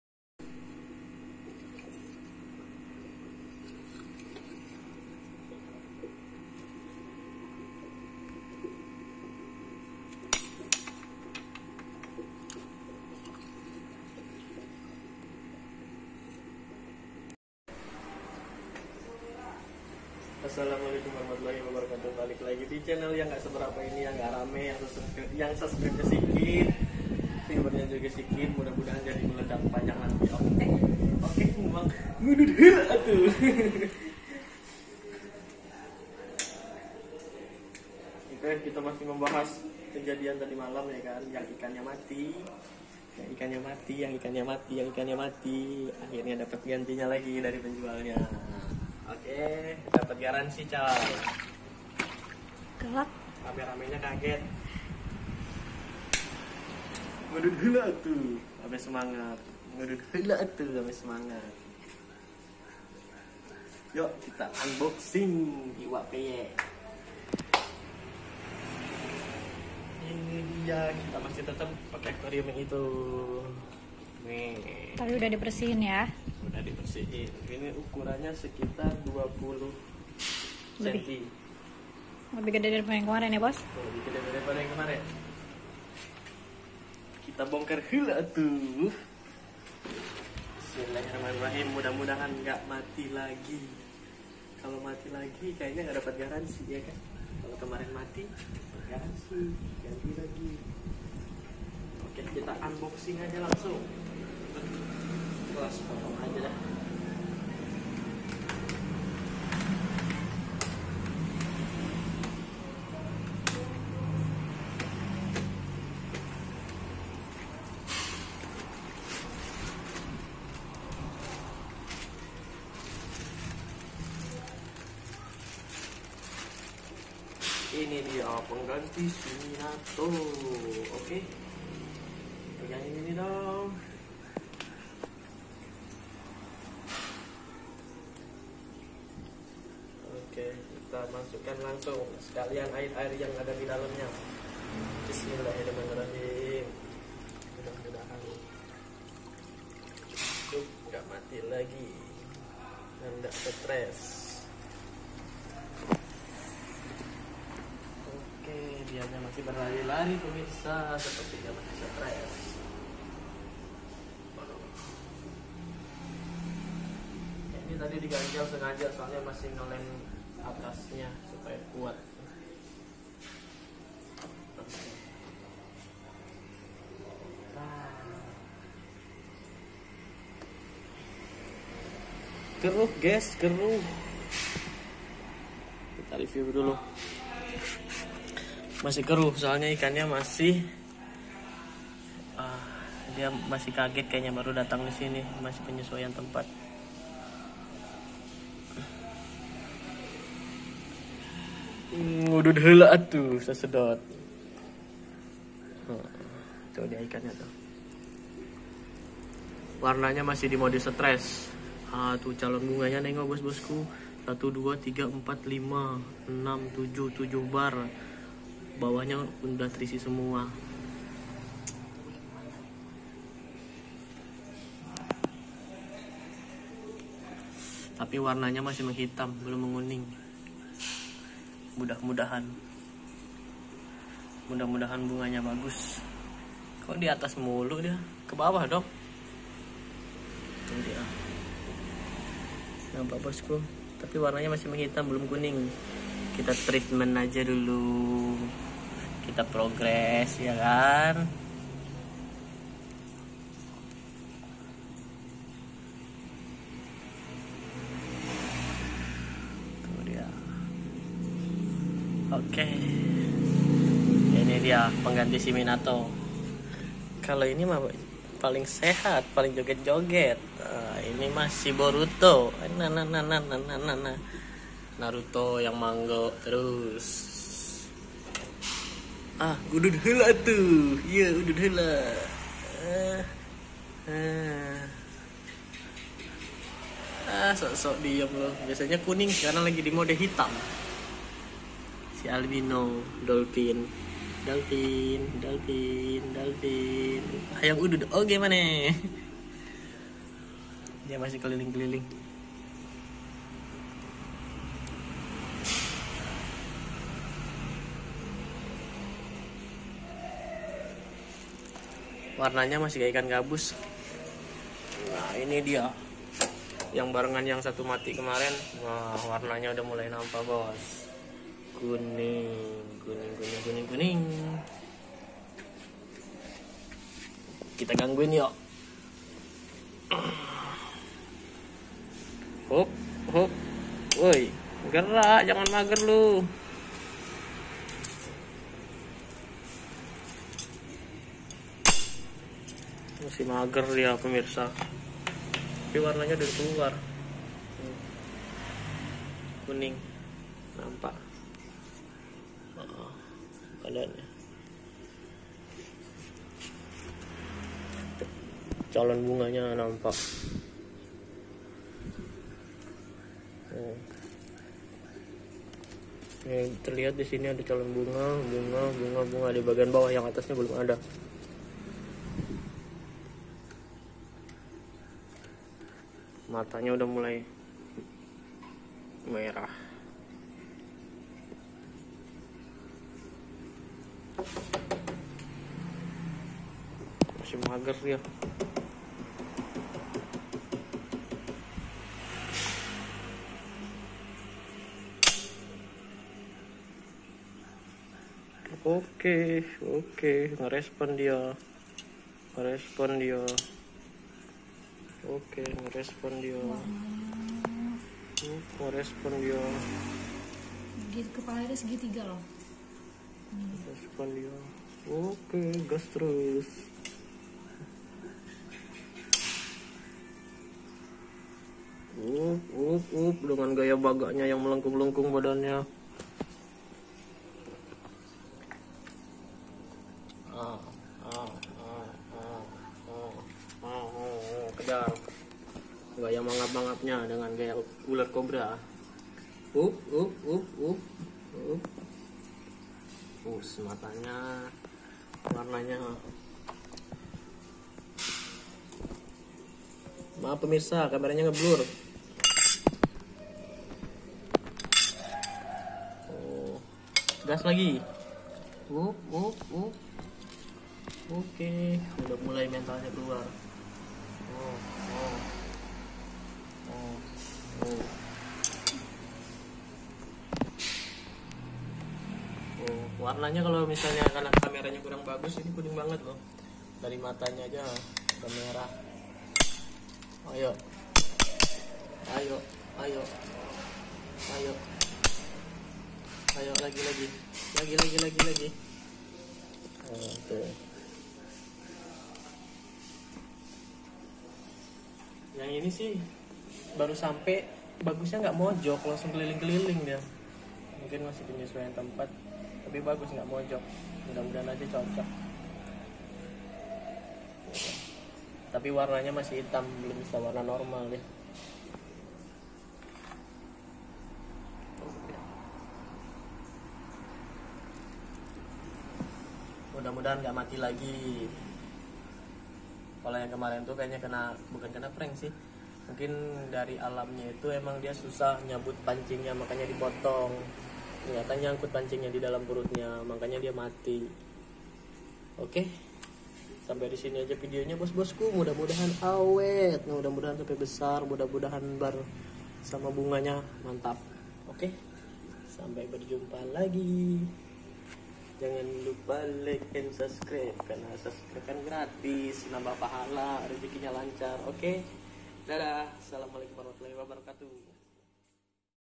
IKAN CHANNA MARU YS GARANSI BARU YANG MATI KEMARIN 25CM
ALHAMDULILLAH,YESSS DAPAT GARANSI BARU IKAN CHANNA YANG MATI KEMARIN